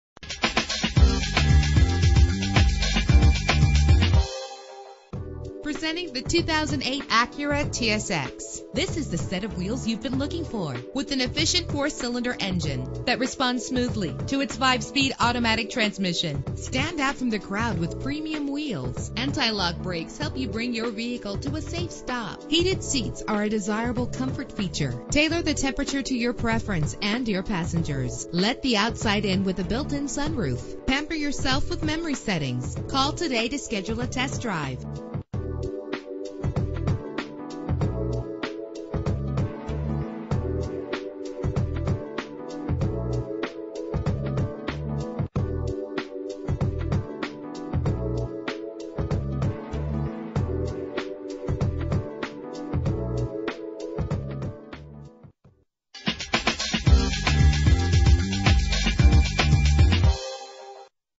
2008 Acura TSX - Bountiful UT
Year: 2008
Make: Acura
Model: TSX
Trim: Base
Engine: 2.4 liter inline 4 cylinder 16 valve
Transmission: 5-Speed Automatic
Color: Gray
Mileage: 38478
Address:
2380 South Hwy 89